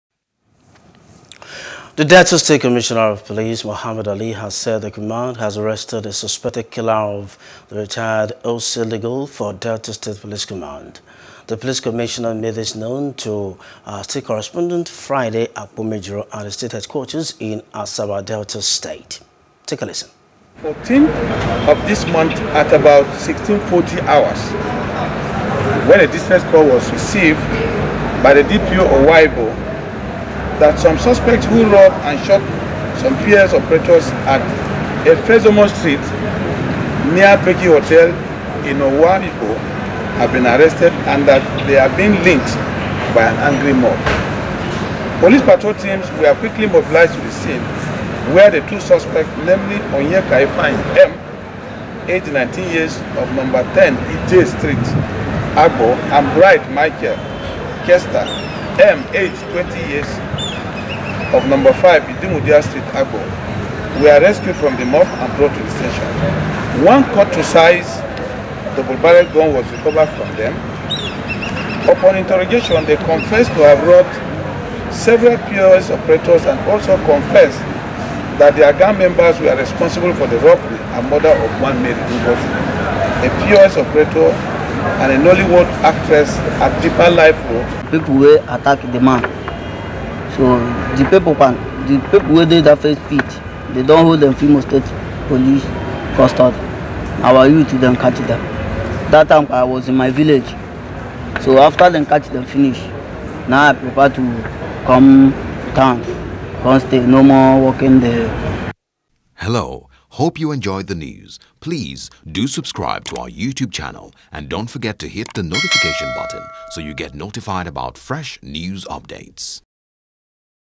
Delta: Police Arrest Murderer Of O.C. Legal/ POS Operator. | NEWS
The Delta State Commissioner of Police Mohammed Ali has said the command has arrested the suspected Killer of the Rtd OC Legal for Delta state police command.